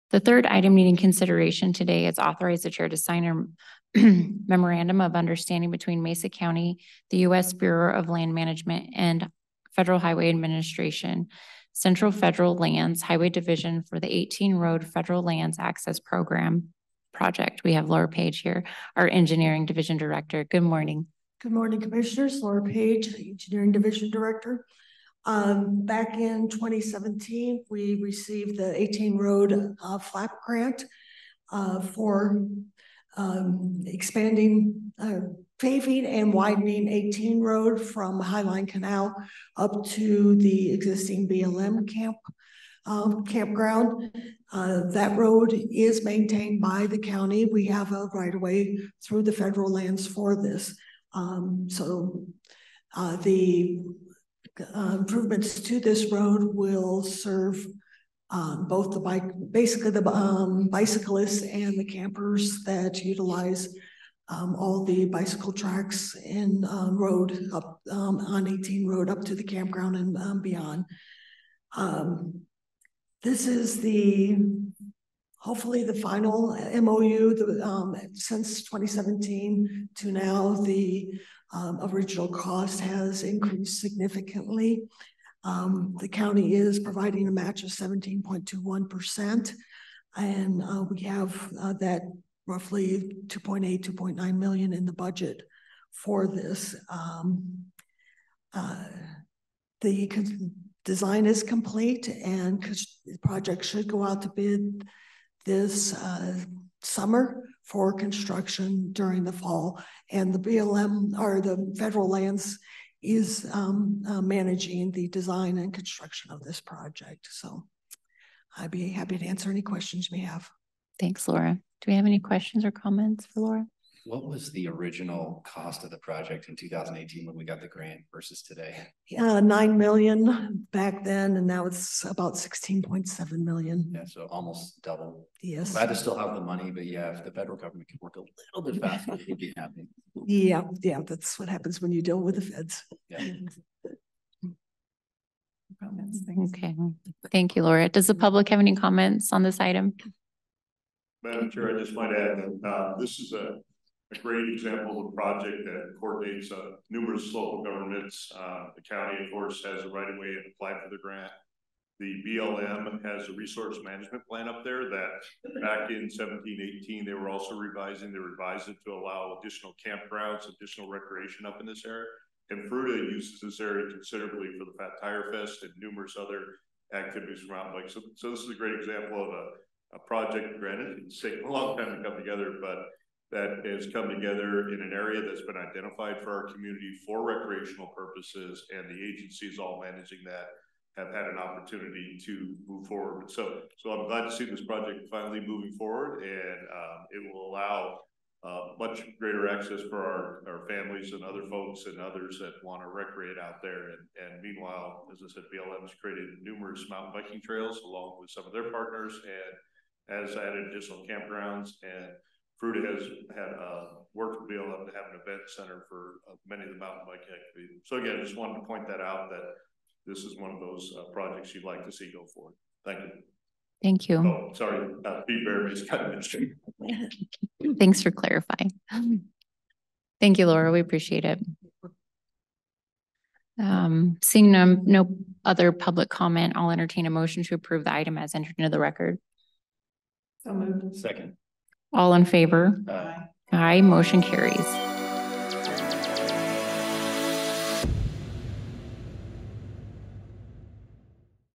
Improvements to 18 Road will offer safer access to recreation area
18 Road will see improvements to provide safe access to the recreational area for bike, foot, and equestrian traffic. At the April 30 public hearing, the Chair of the Board of County Commissioners signed a Memorandum of Understanding (MOU) between Mesa County, the U.S. Bureau of Land Management, and the Federal Highway Administration — Central Federal Lands Highway Division to start the process of the 18 Road Federal Lands Access Program (FLAP) project.

The project aims to pave and widen 18 Road from Highline Canal to the Colorado Parks and Wildlife Campground to a 32-foot width, including four-foot paved shoulders on each side for bike, pedestrian, and equestrian use.

Mesa County was awarded a grant for the 18 Road Federal Lands Access Program project in 2017, and this MOU identifies and assigns responsibilities for the environmental analysis, design, right-of-way, utilities, acquisition, construction, and potential future maintenance for the project.

Mesa County will match approximately 17% of the project’s total cost, estimated at $16.7 million.